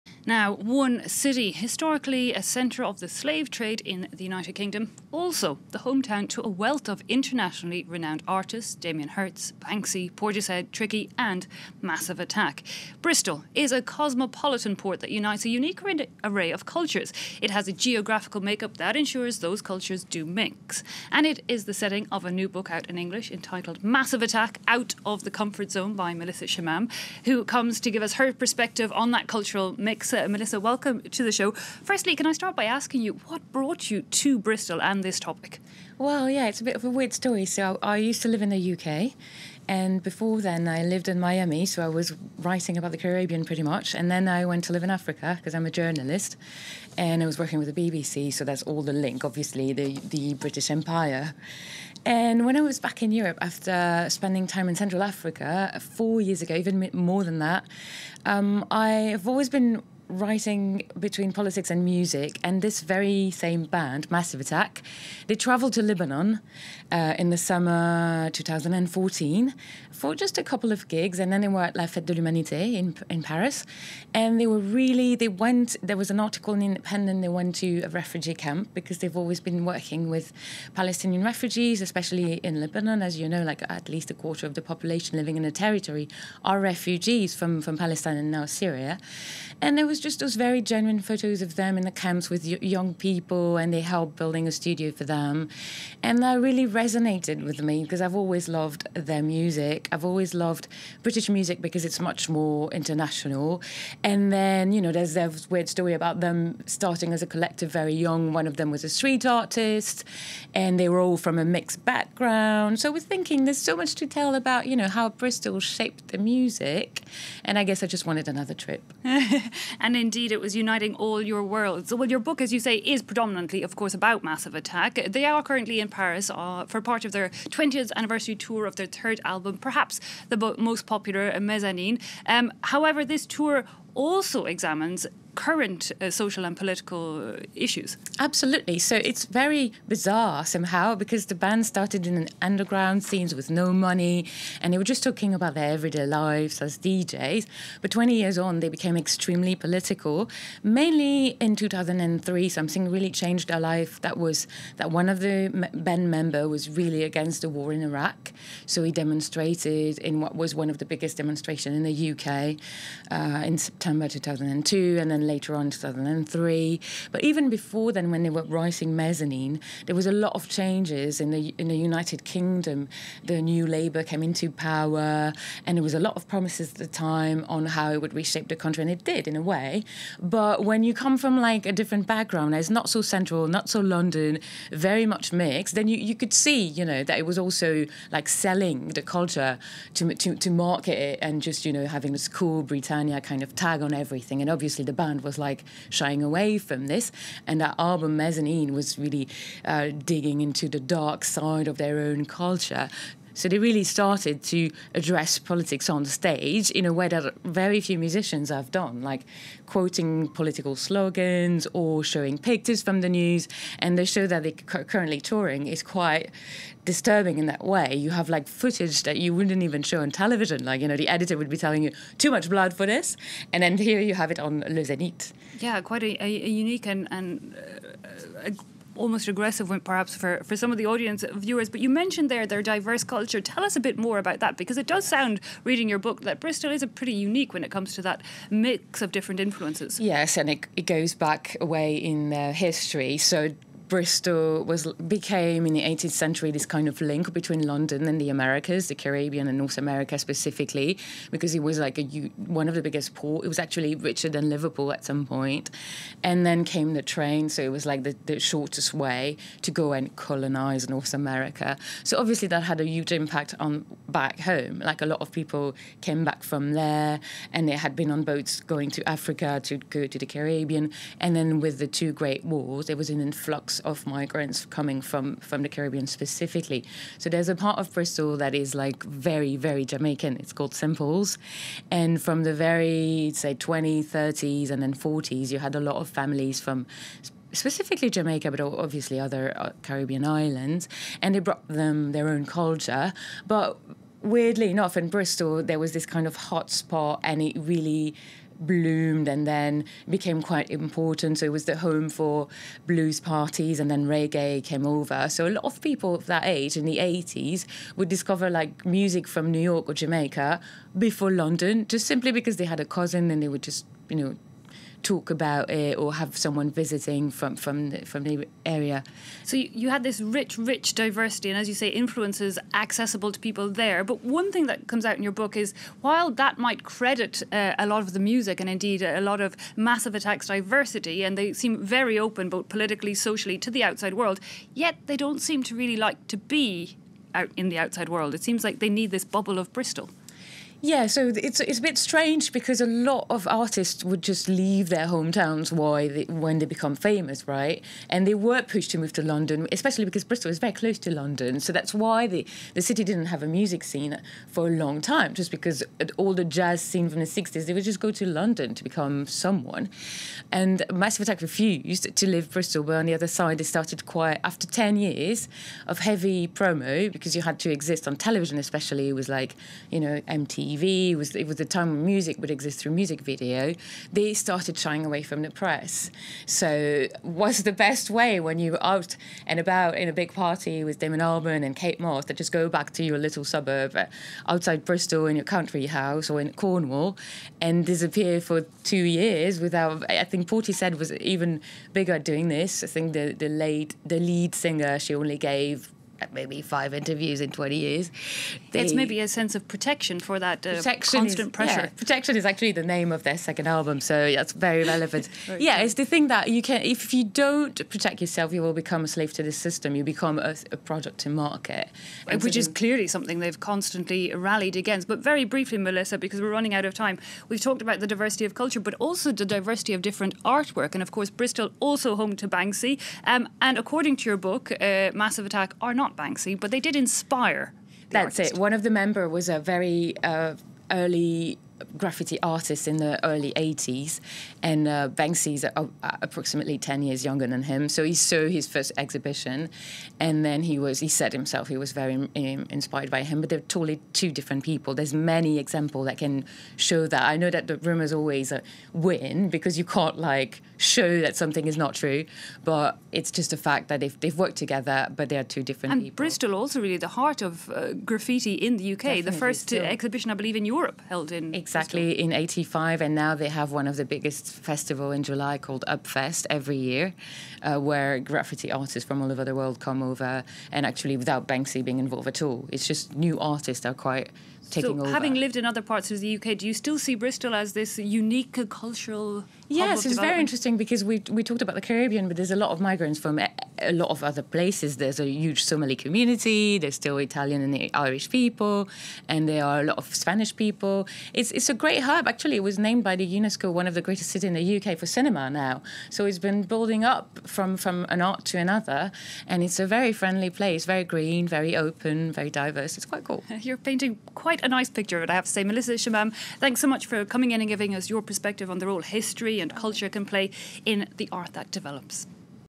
How Bristol became a musical and artistic melting pot
Historically a centre of the slave trade, Bristol is the hometown of a wealth of internationally renowned artists: Damien Hirst, Banksy, Portishead, Tricky and Massive Attack. Author Melissa Chemam says that there is a clear link between those two facts. As people were brought in from across the world, their cultures came with them. The result put Bristol ahead of much of the UK when it came to new and different sounds and ideas.